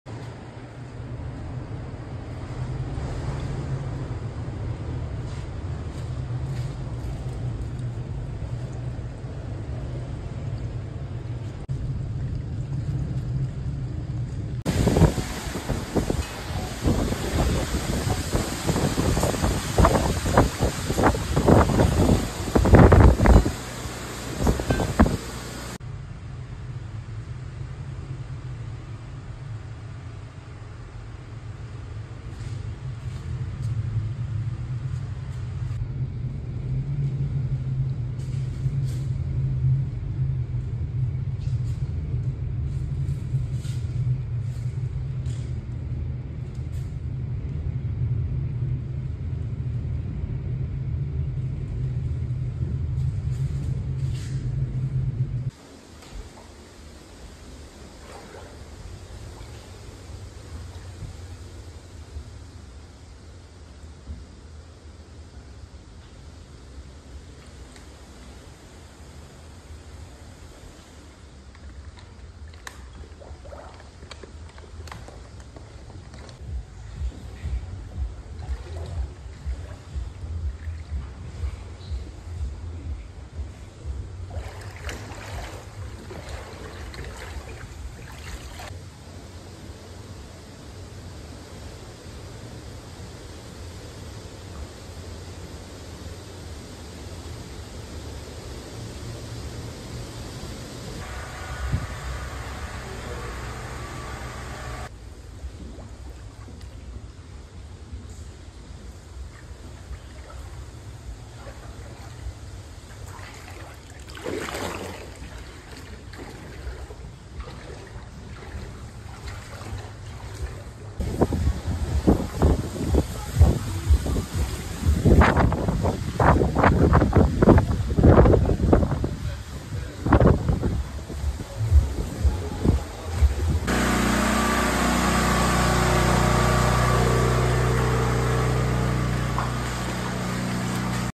Hurricane Helene Turns Living Room Into Swimming Pool
A tree crashed onto a roof of a home as floodwaters from Hurricane Helene inundated a property in Bradenton, Florida, on Saturday, September 28.
Credit: Skylar Siegfried via Storyful